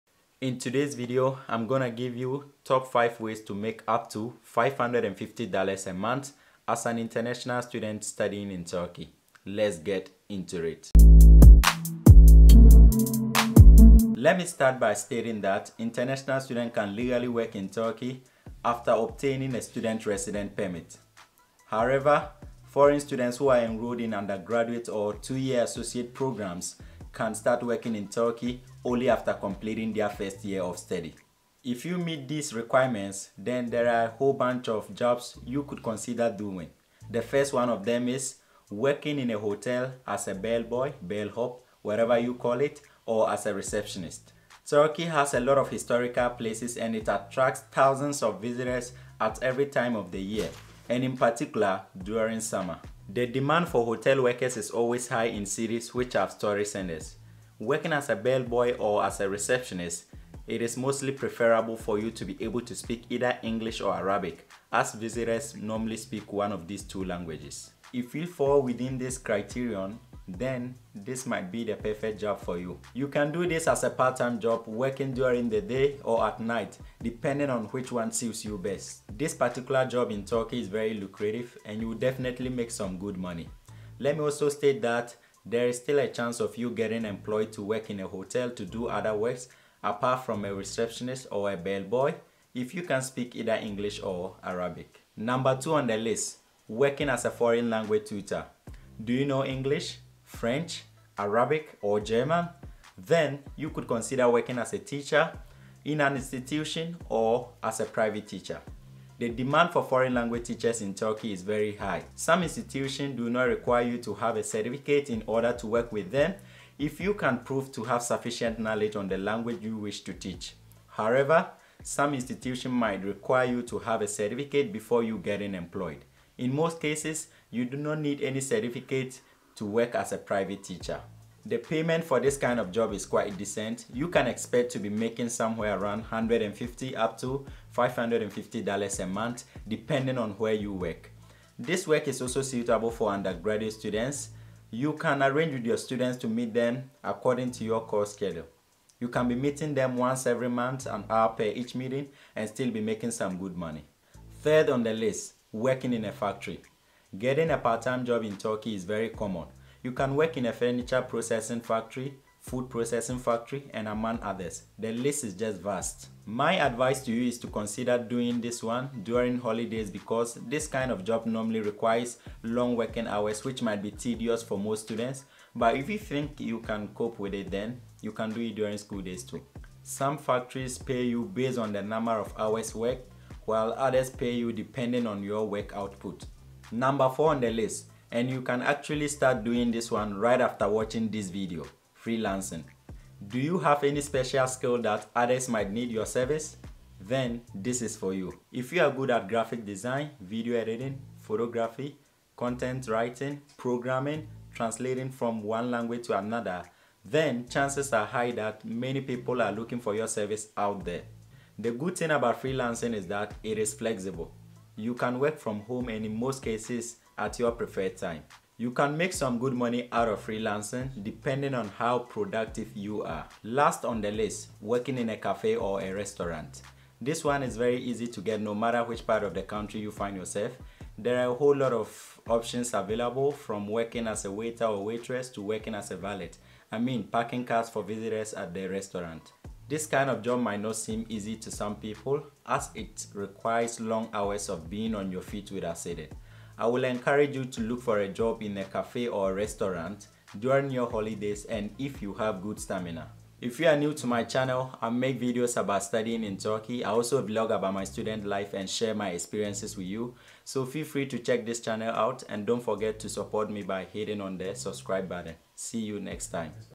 Top Part-Time Jobs in Turkey for International Students||550$ a month🤑
Turkey has become a top destination for studies for most students around the globe. In this video, I listed and explained how to make 550$  or more every month as an international student in Turkey.